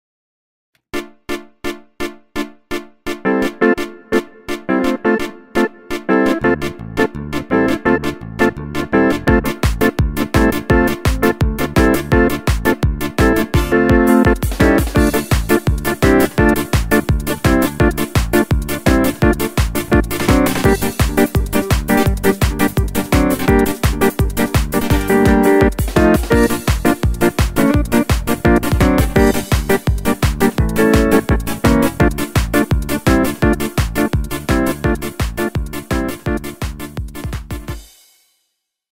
KORG DB SET 2018 - Maco Mamuko Style
KORG set DB 2018 - obsahuje približne 60 štýlov a množstvo zvukov štúdiovej kvality (piána, fender piána, akordeóny, cimbaly, gitary, basy, bicie sady, audio bicie, multipady)

Set bol pripravený pre hráčov všetkých žánrov s obrovským využitím zvukov a štýlov ako do štúdia, tak aj na live hranie

The set was prepared for players of all music styles with a huge usage of sounds and styles as in studio or live playing.

It contains appr. 60 styles and a plenty of sounds of studio quality (piano, fender piano, accordions, cimbals, guitars, basses, drums, audio drums, multipads)